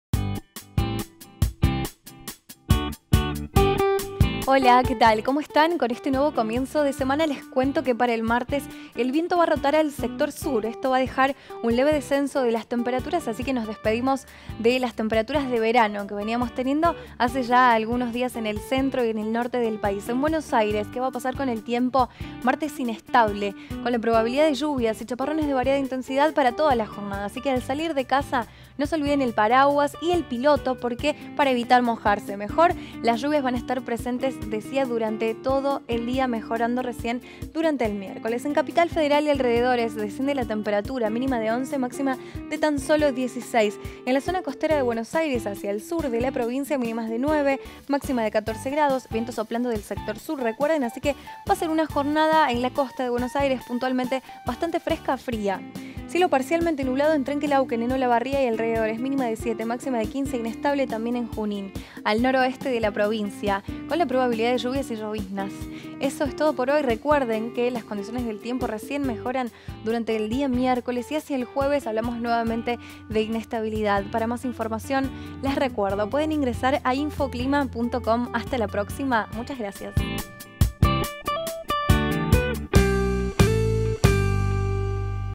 Pronóstico para el 08 de agosto de 2017. Argentina - Infoclima TV
Presenta: Sofia Garone